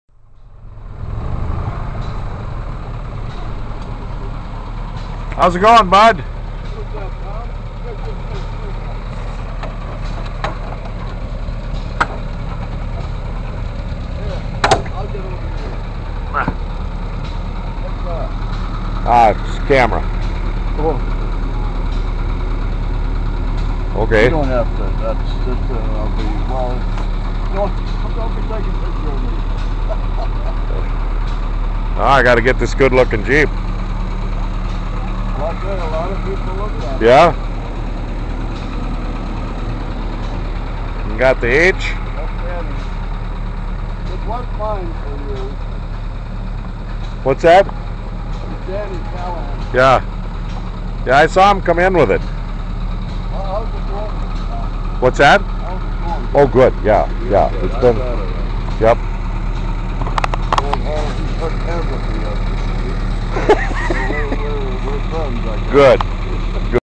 Bud Callahan at 2009 LeSueur County Pioneer Power Show
Bud with his 1940's military jeep